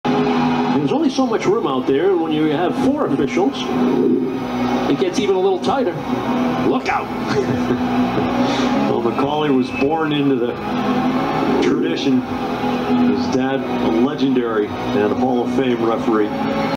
NHL Referee Wes McCauley bumped and dumped
Referee McCauley takes a check in the far corner and goes down in style. He was uninjured and play continued. Sabres at Bruins, 16 Dec 2018.